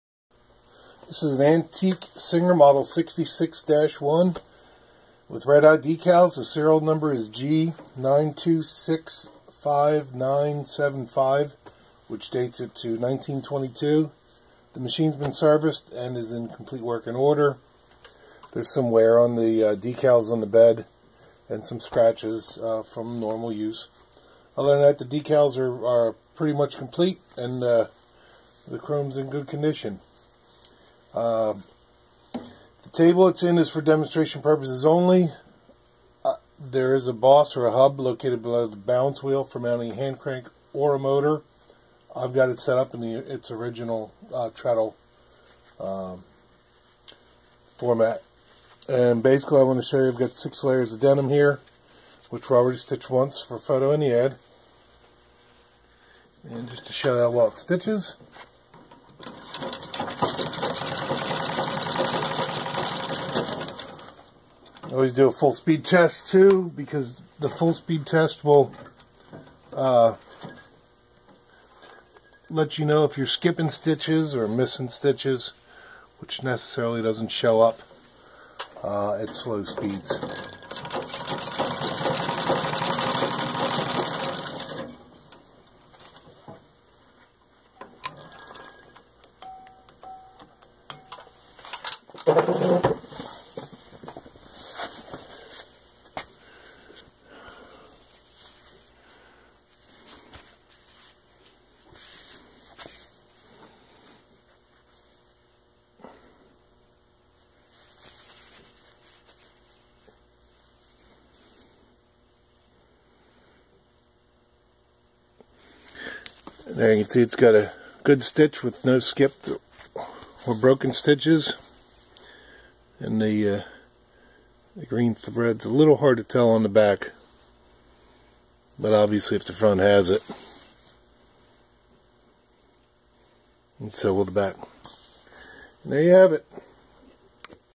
Serviced Antique 1922 Singer 66 Red Eye Treadle Sewing Machine Upgraded Side Clamping Foot G9265975
This video demonstrates a Serviced Antique 1922 Singer 66-1 Red Eye Treadle Sewing Machine serial number G9265975.
This machine has been upgraded with a side clamping foot!
The treadle base is NOT included with the machine. It is used for demonstrating the machine at operating speed.